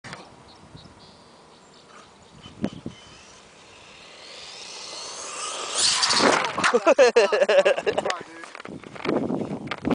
Mini E Revo Smashes Buddy
This was our first blooper while filming..lost my line..lol..hopefully we can get a nice complete film after we are done..but for now enjooy